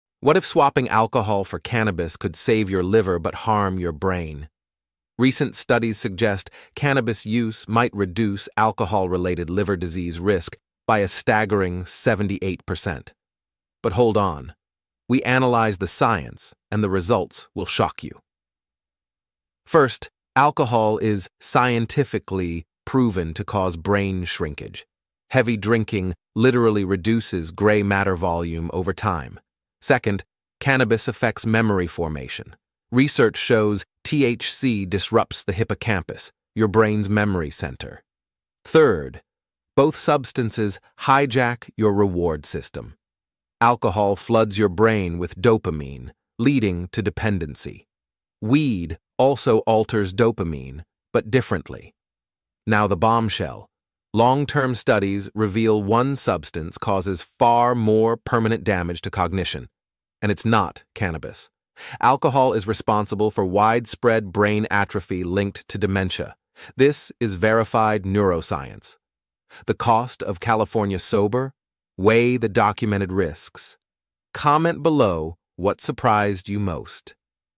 California Sober Brain: The Shocking Truth
New research shows swapping alcohol for weed may cut liver disease risk by 78%. But cannabis shrinks your brain? We reveal the VERIFIED science on how each substance affects cognition.